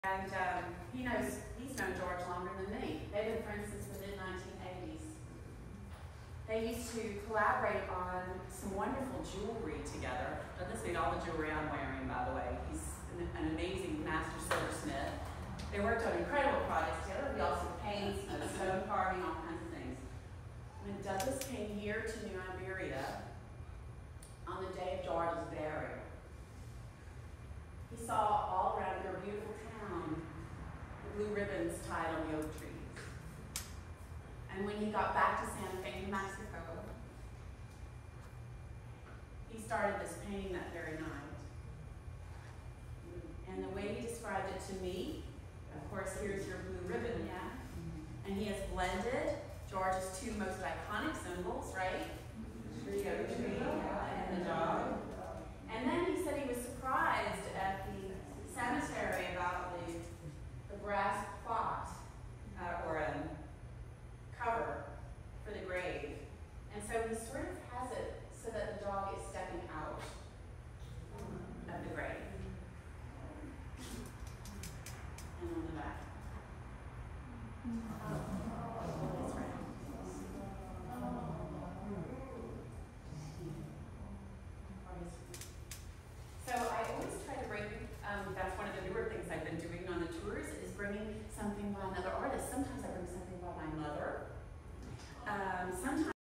“RIP George Rodrigue” A Painting by Douglas Magnus
During a special presentation for the 100th Anniversary of the Louisiana Fortnightly Literary Club, Wendy Rodrigue shares a painting by her current husband, Douglas Magnus, in honor of her late husband, George Rodrigue (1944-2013). 💙

Filmed by Douglas Magnus at the Bayou Teche Museum in George Rodrigue’s hometown of New Iberia, Louisiana. 💙